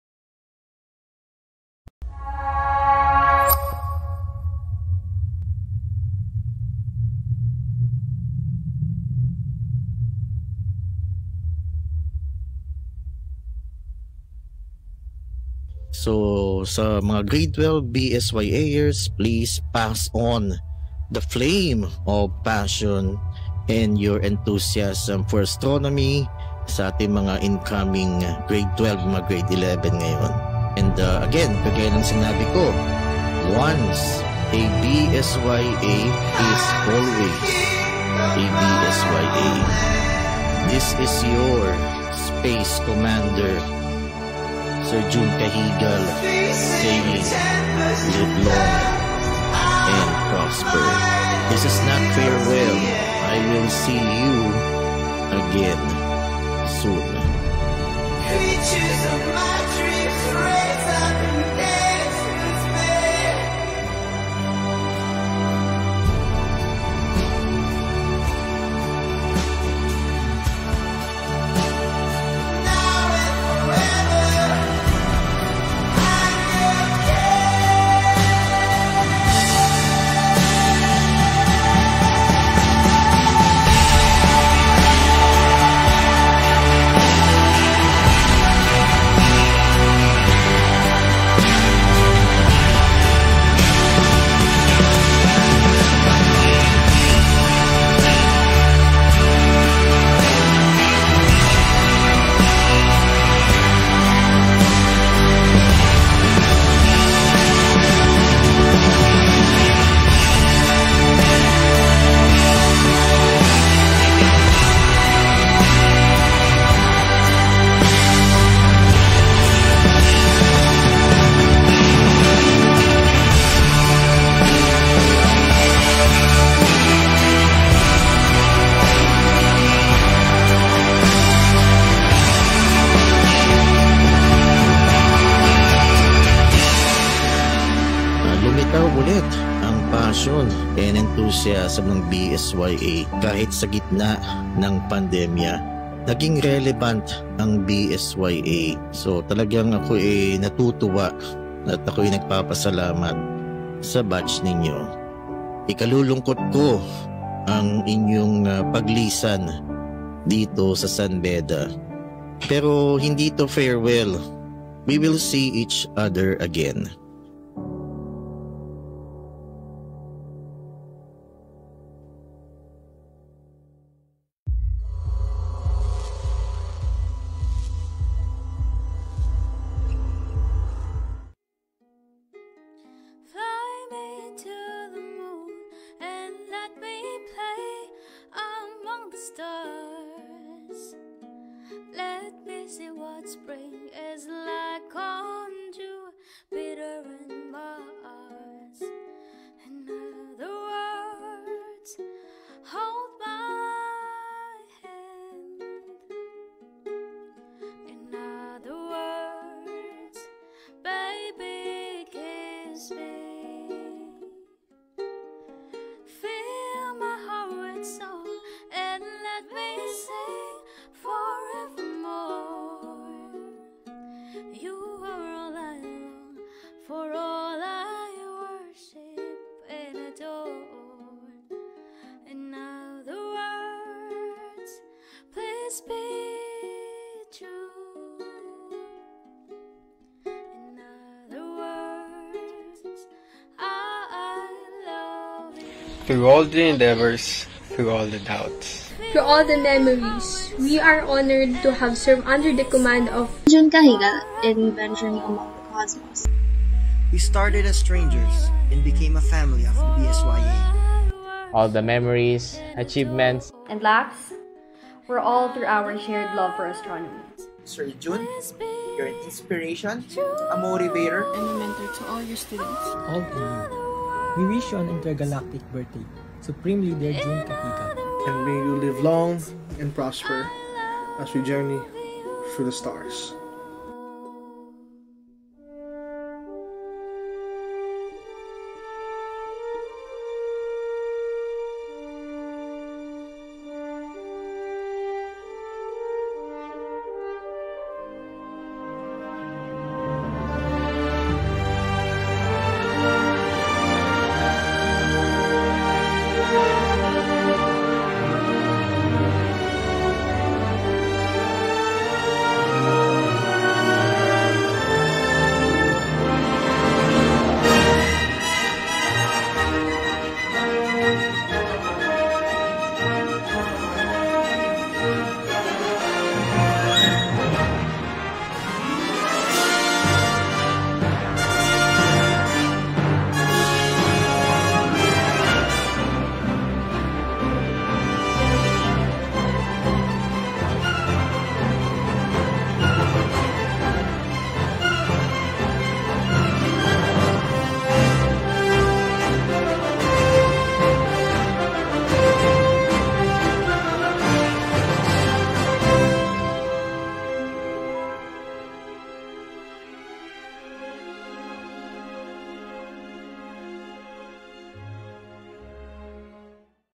BSYA NOW AND FOREVER A Tribute to the Outgoing Officers of BSYA SY: 2020-2021
School Year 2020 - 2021, Covid-19 hit and crippled the whole world. We all went through hard times, depression, deaths. But in spite of the limitations, we were able to conduct ALL our activities “VIRTUALLY”. We learned from each other more than we thought we knew. We shared laughter, played the "AMONG US" online game, late-night meetings via DISCORD, conducted online webinars/lectures and virtual telescope viewings, and most of all we have become relevant with the pandemic. We’ve learned so much wisdom. Thank You to BSYA Batch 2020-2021 for the memories. This video is for all of us. We are in these hard times together, one batch- THE QUARANTINE BATCH, 2021. video credit-Cuz Paculanan and BSYA officers and crew Batch 2021